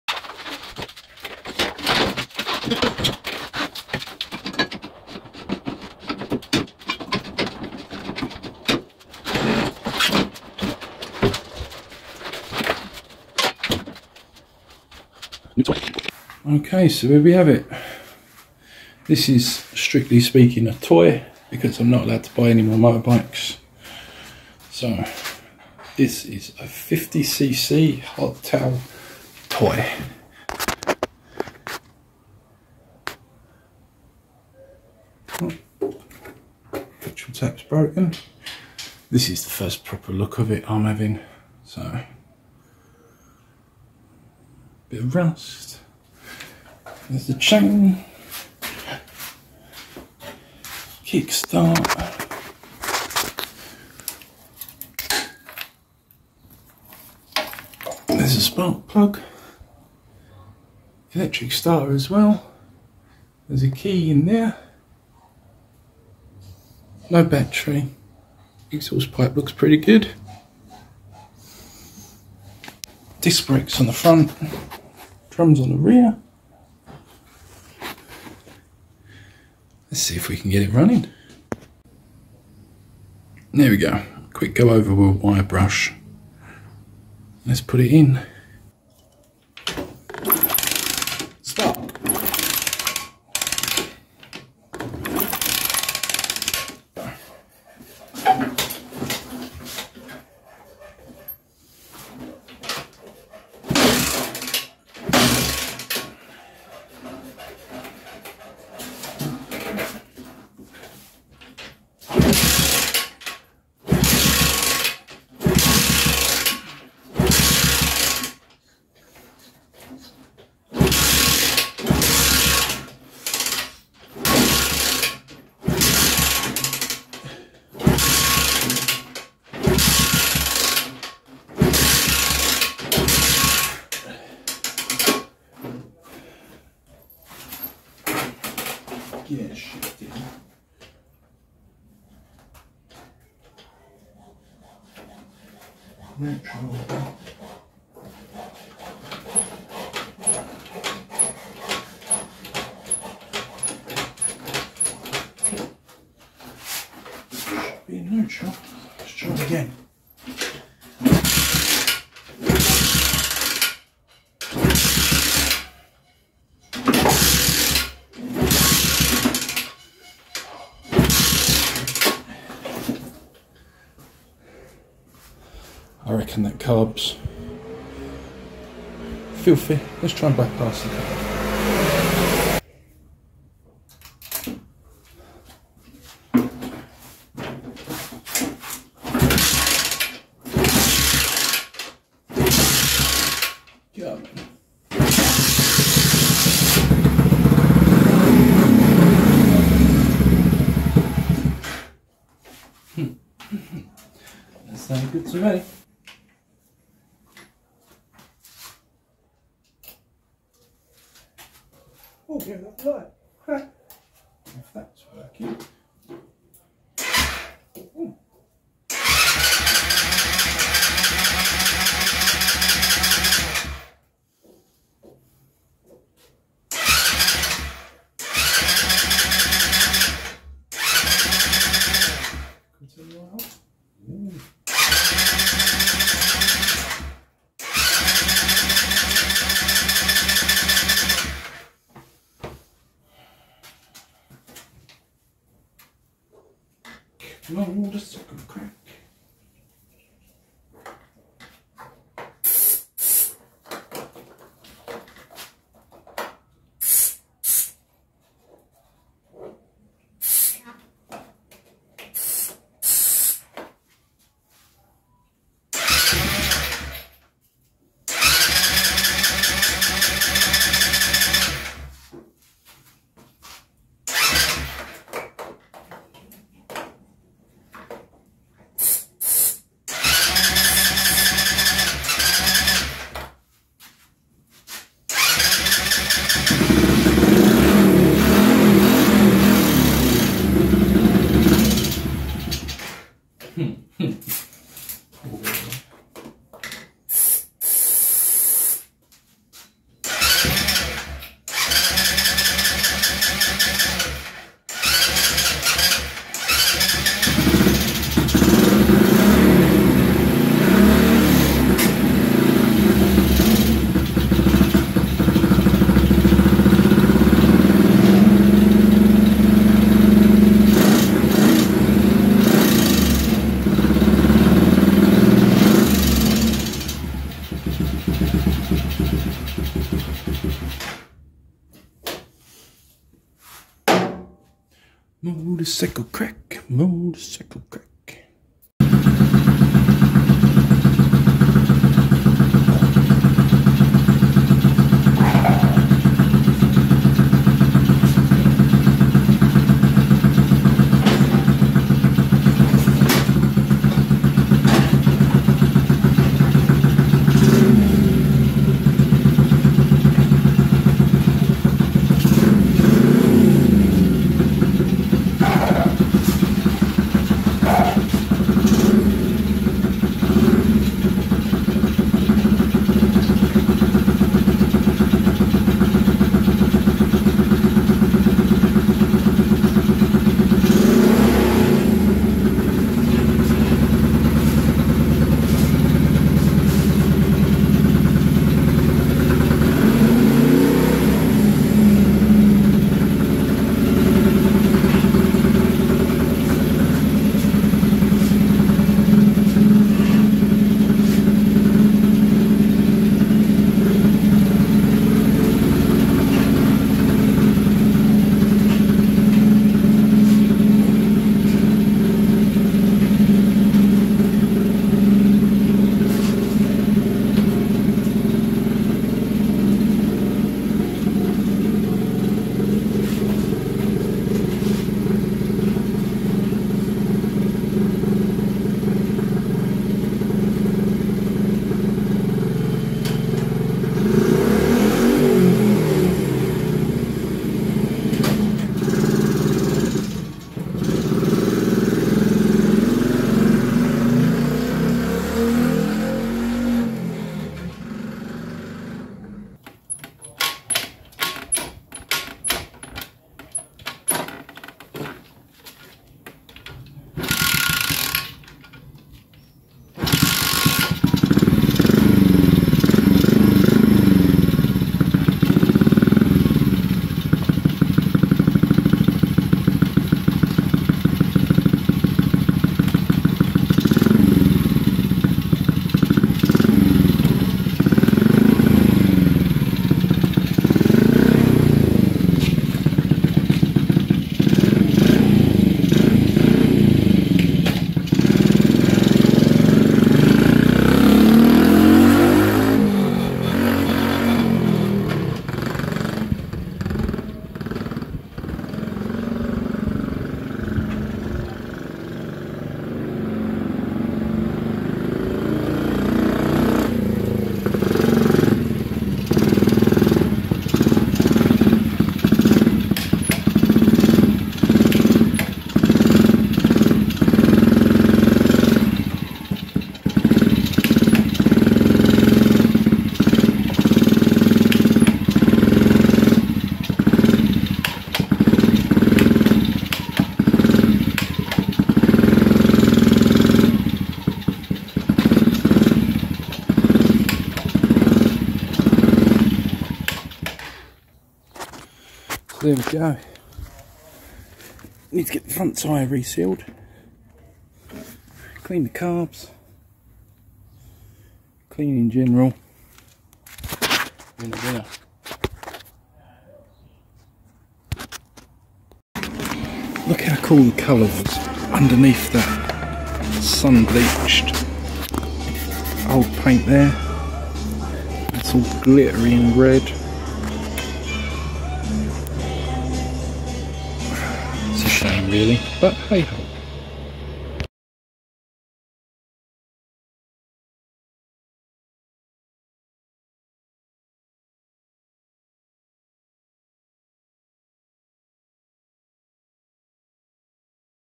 MINI CHOPPER RESCUE - WILL IT RUN?
THIS IS A TOY HONEST
KIDS 50CC GONNA GET RUNNING AND HAVE SOME FUN WITH IN THE FUTURE